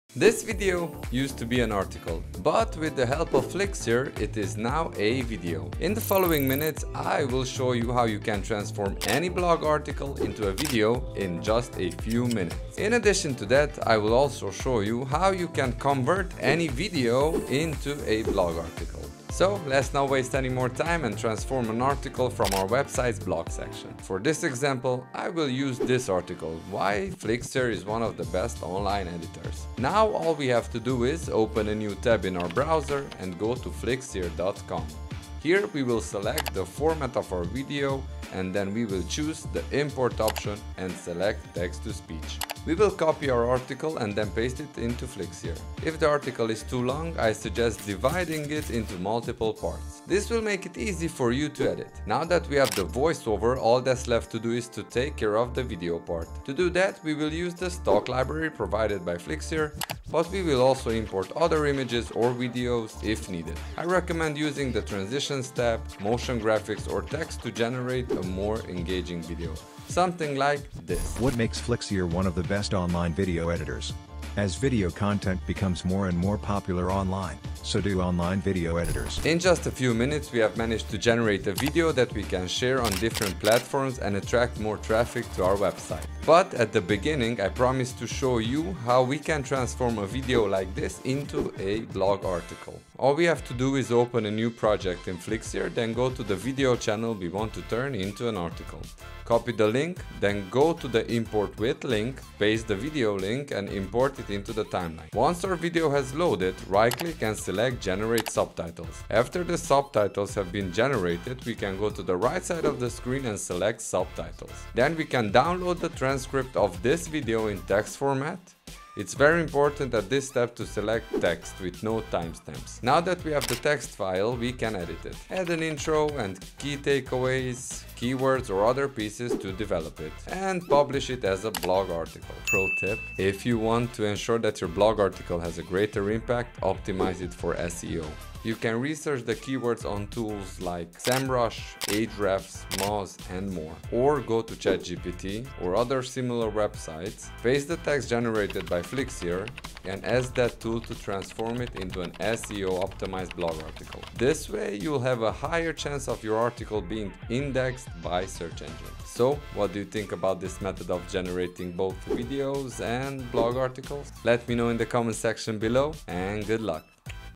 How To Turn Articles Into YouTube Videos With Flixier
Learn how to effortlessly convert articles into captivating YouTube videos using Flixier! Watch this quick tutorial and discover the power of repurposing your written content for a wider audience. Create professional-looking videos in minutes with Flixier's intuitive editing tools. Start optimizing your content strategy today!